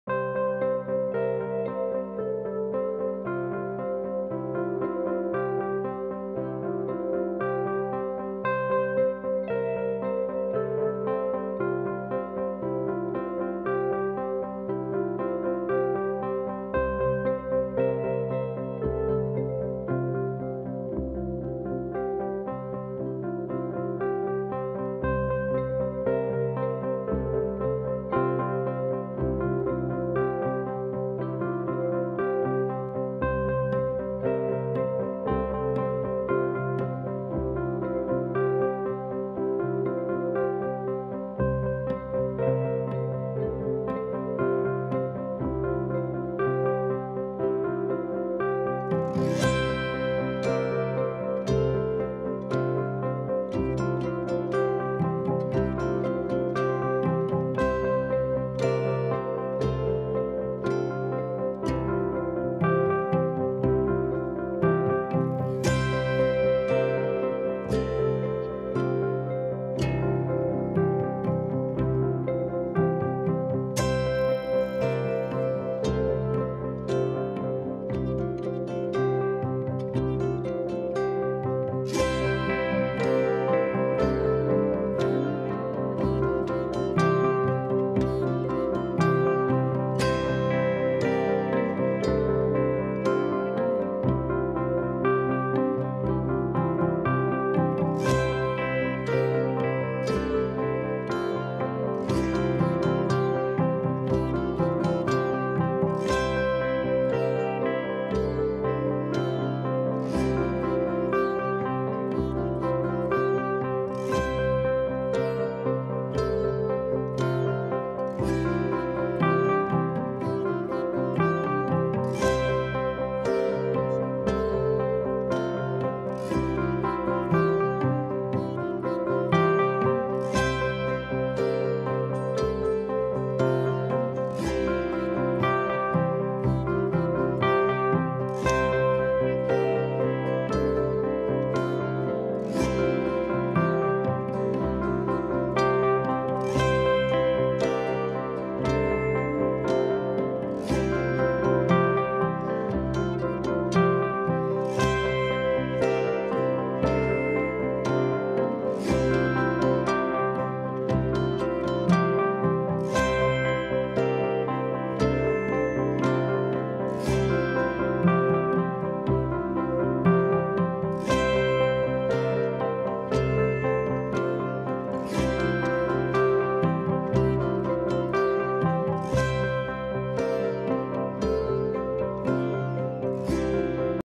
freestyle line art illustration
I will be sharing with you guys how to draw art illustrations.
Here I have used sakura pigma fineliner